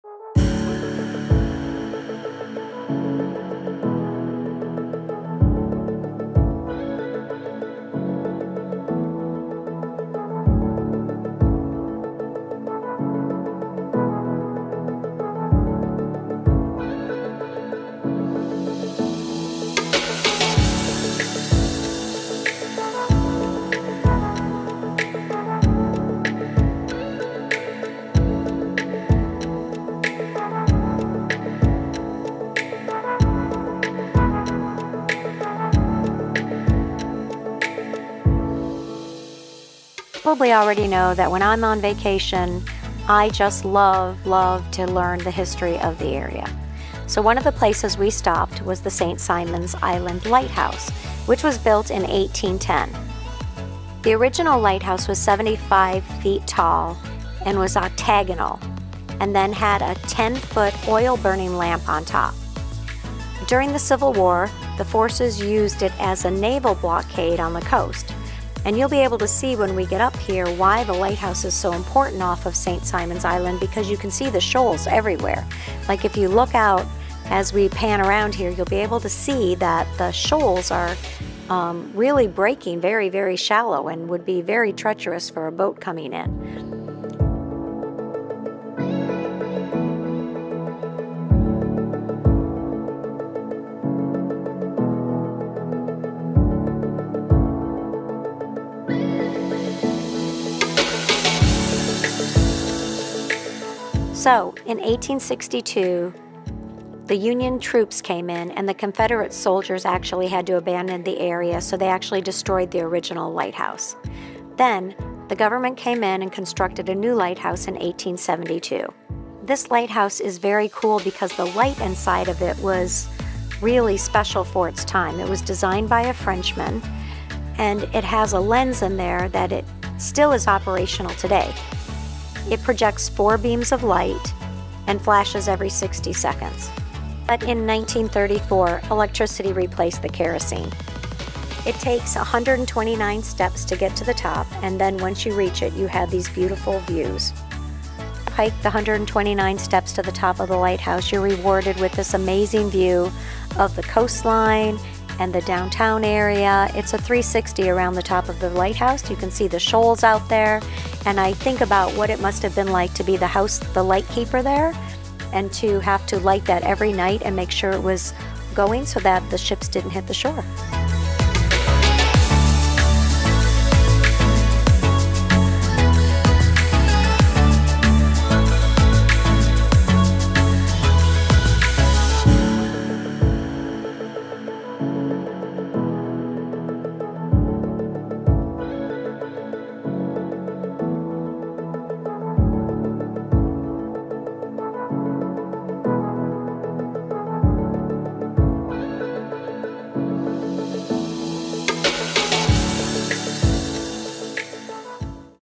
Visiting St Simons Island LIGHTHOUSE MUSEUM, Georgia Golden Coast
This a complete documentary vlog of the Georgia golden coast. we visited six hotels and numerous beaches, restaurants, and historical places while we were in the Georgia Golden Coast area on the Jekyll Island & St Simon Island.
Coming up: Sea Island Beachfront Resort, The Cloister at Sea Island, and The Lodge at Sea Island Golf Club.
Then we’ll take you over to The King and Prince Beach & Golf Resort and also show you the Hotel Simone.
We then jump over to Jekyll Island, just a short drive away, to see the Jekyll Island Club Resort and The Westin Jekyll Island.